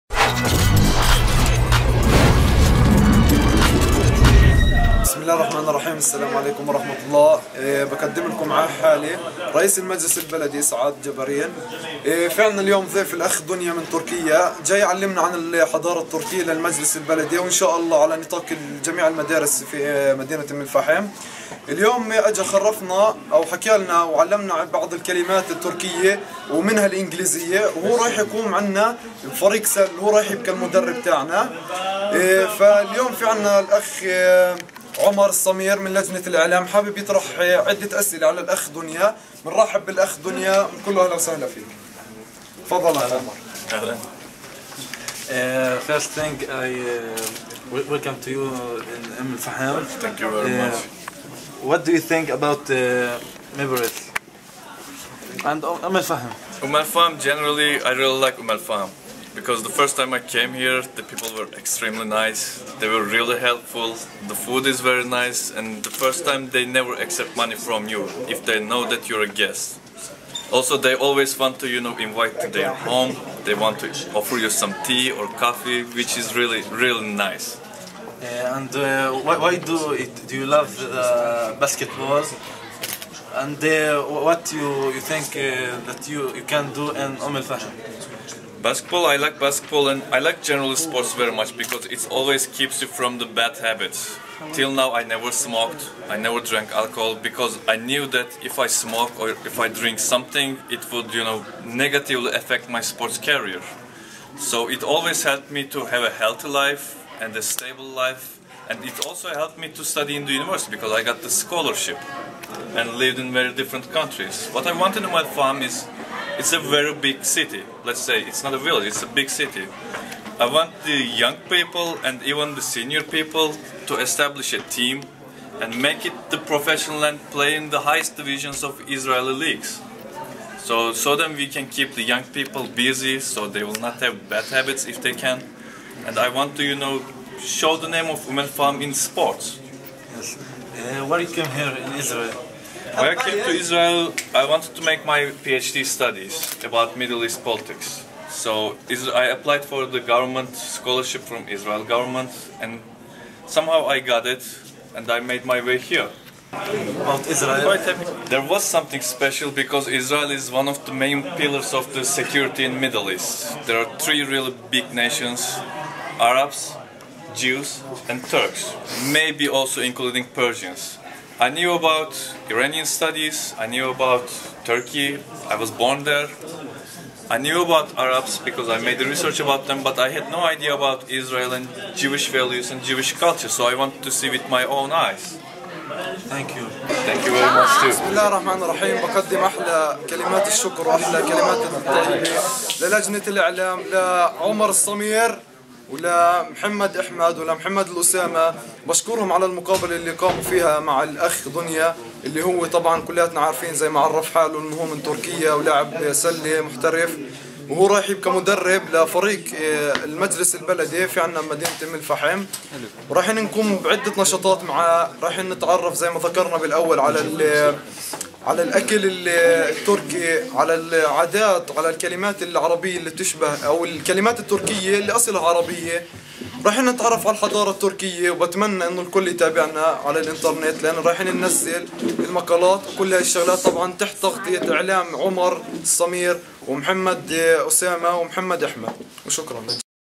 مجلس الطلاب البلدي في أم الفحم-موقع المسار
مجلس الطلاب البلدي في أم الفحم يستضيف محاضرا من تركيا. تابع الخبر عبر موقع وصحيفة المسار